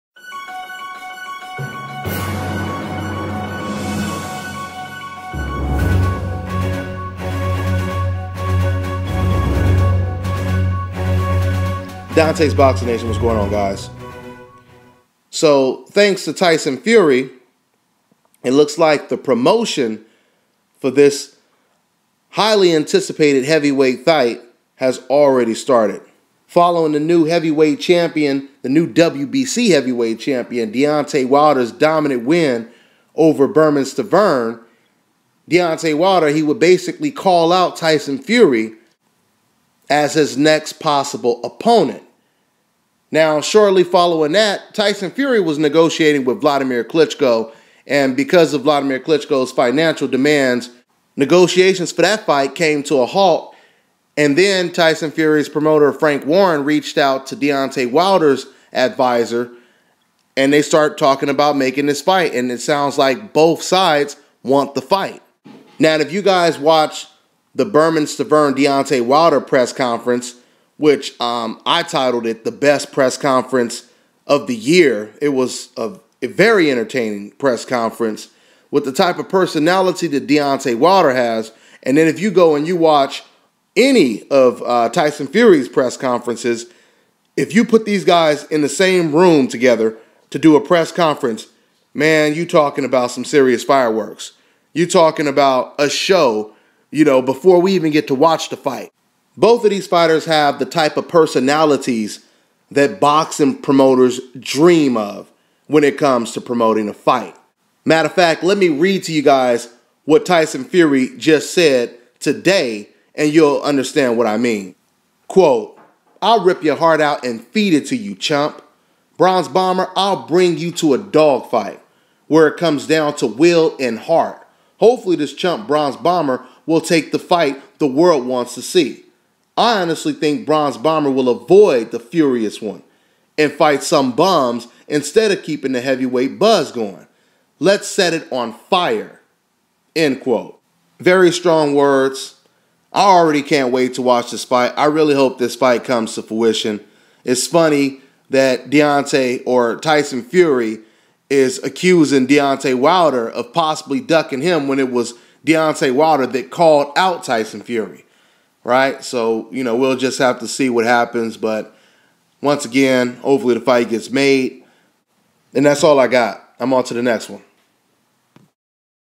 TYSON FURY SENDS A VIOLENT MESSAGE DIRECTLY TOWARDS DEONTAY WILDER!!
Things are heating up as Tyson Fury sends direct violent message to Deontay Wilder!!